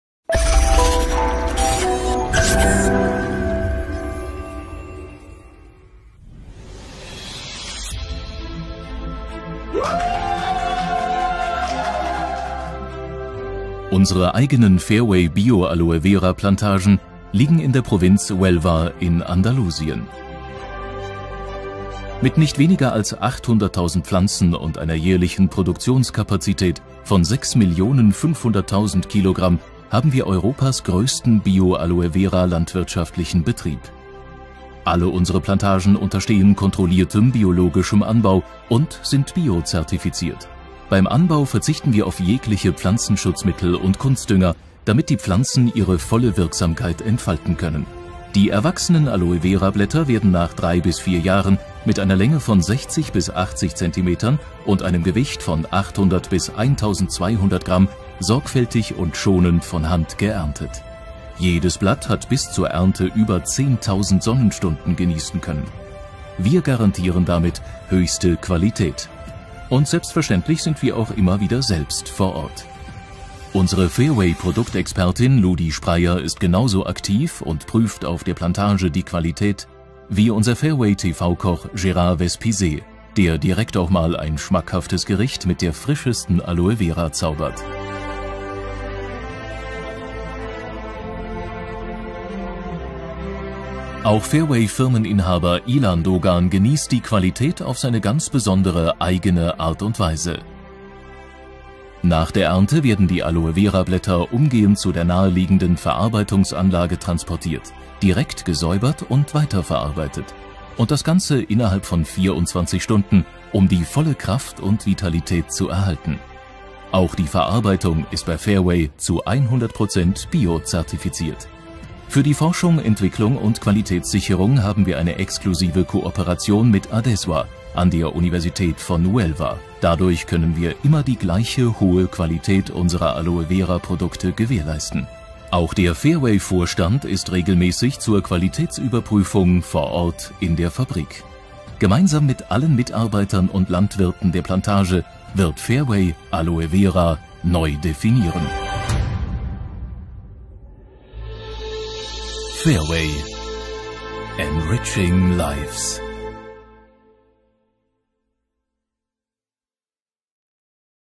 VERWAY La plantacion mas grande en Europa de Bio Aloe Vera en Huelva, España
La plantacion mas grande en Europa de Bio Aloe Vera esta en Huelva, Espana y pertenece a la Empresa alemana Verway

Más información, registro y asesoramiento gratuito aquí

 ◾ ◾ ◾ ◾ ◾ ◾CONECTA CONMIGO◾ ◾ ◾ ◾ ◾ ◾

WatsApp:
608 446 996